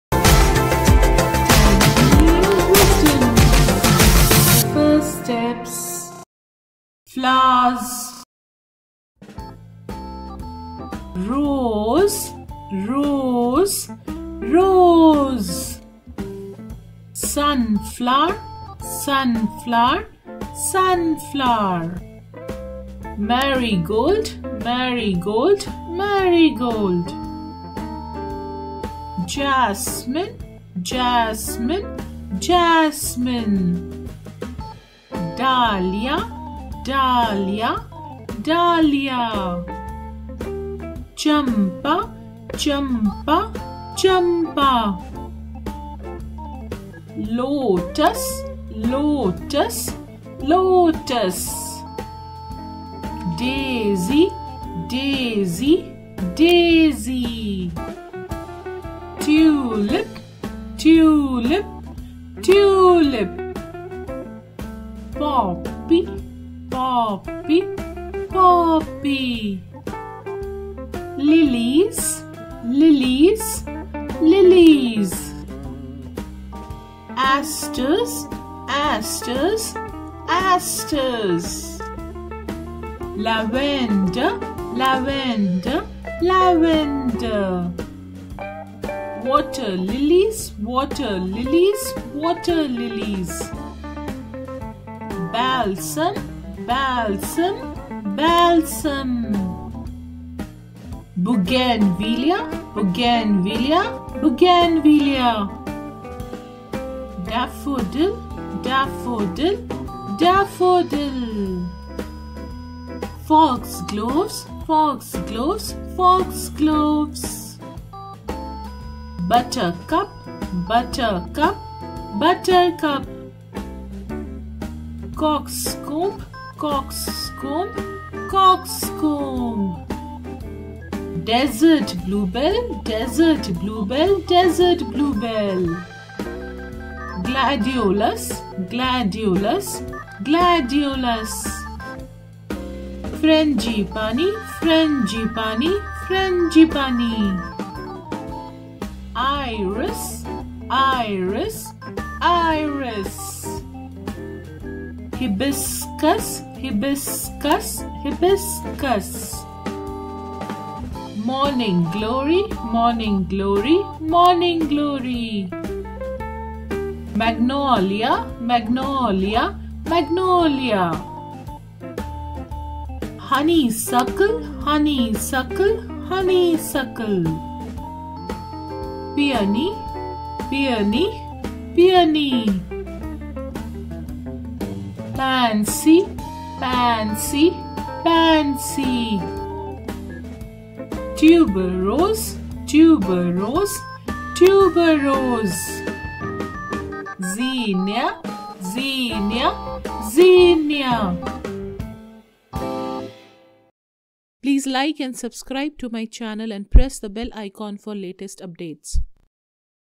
flowers name for kids
This video shows common flowers names along with pictures of flowers. It helps children learn name of  flowers. It builds child's vocabulary.